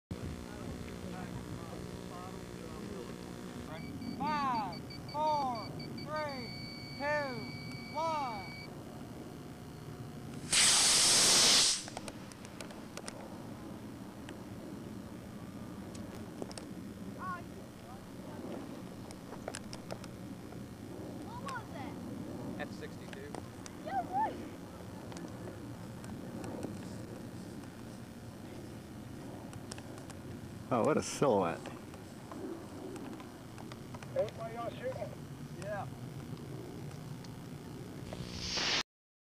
North Coast Rocketry Patriot
North Coast Rocketry Patriot on an F-62-4 Darkstar motor.
(ignore the first few seconds)